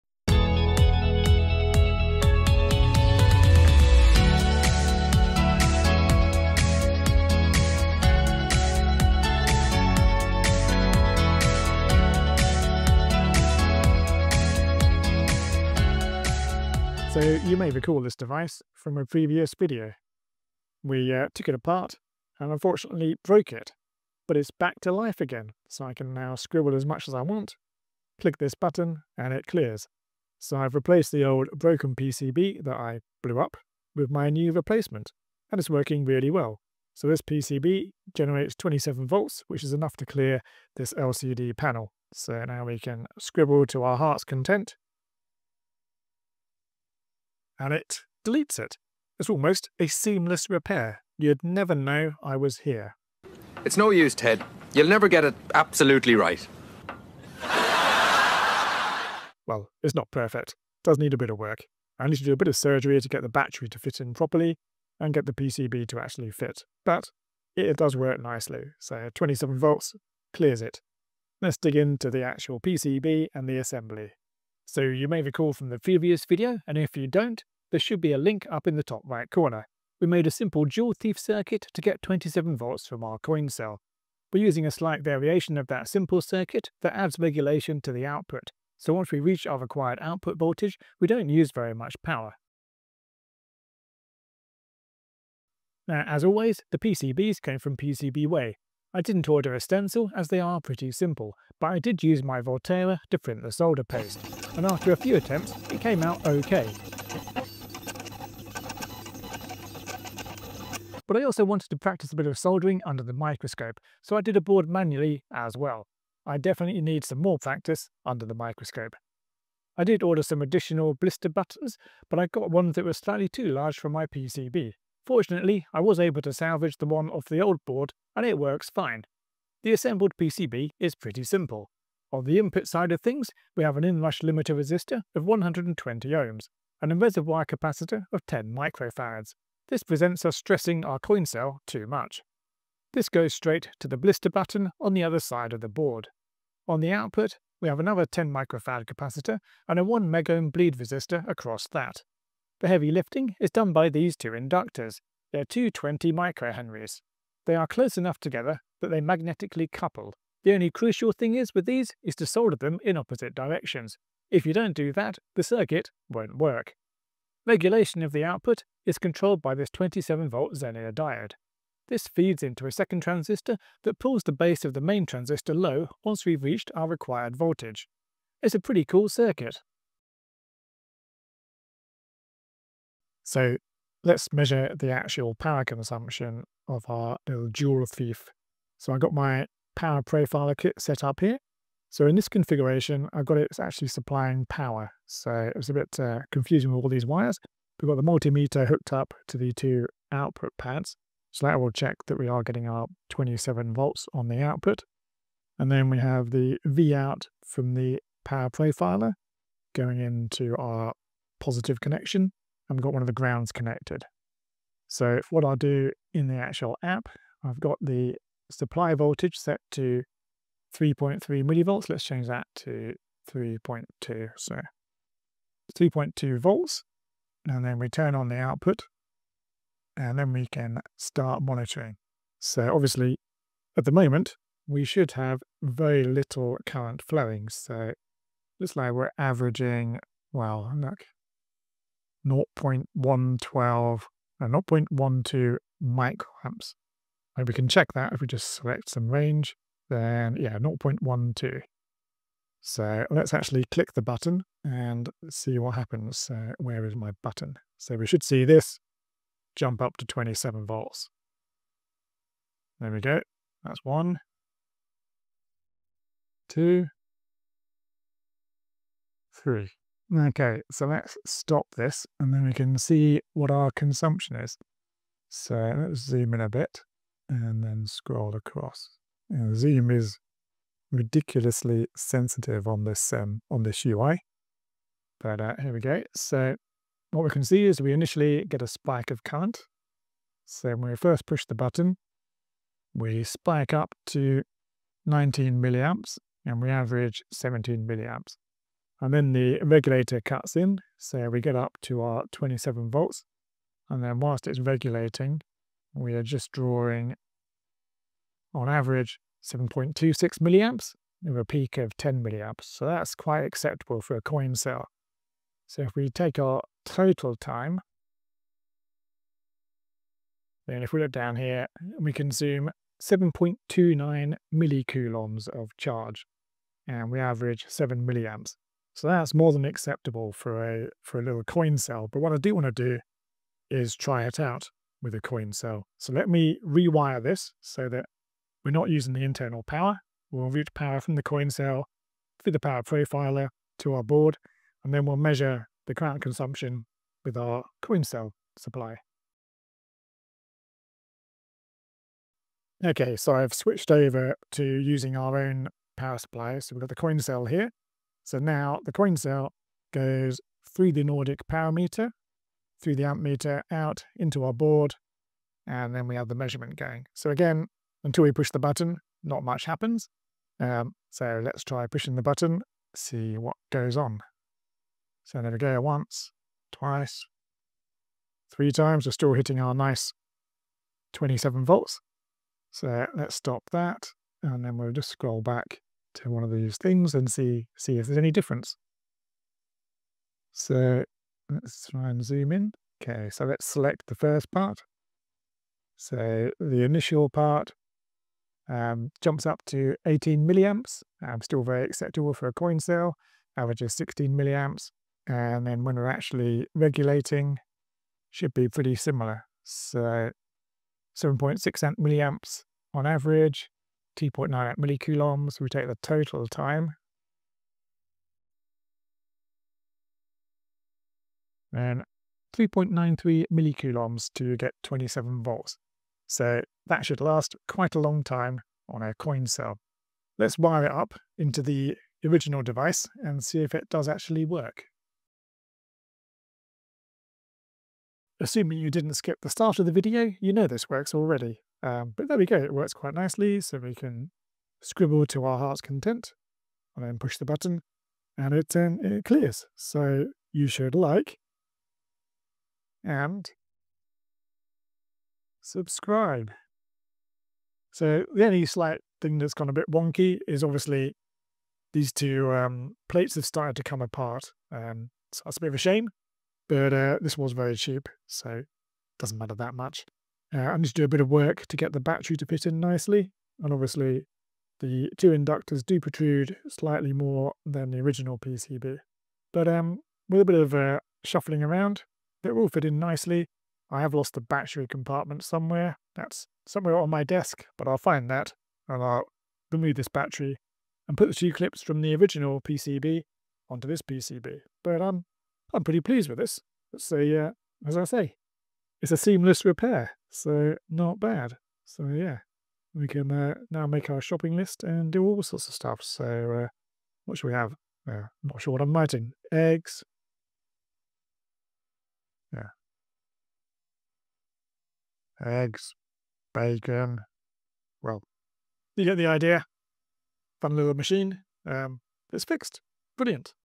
I Built a 27V PCB to Fix This $3 Display...
for the fantastic PCBs and SMT assembly.

I broke my nice display in the previous teardown video - I fixed it with a hacked together Joule thief. I've now got some nice PCBs that should fit in nicely.

You can buy the displays here - shop around as there are many sellers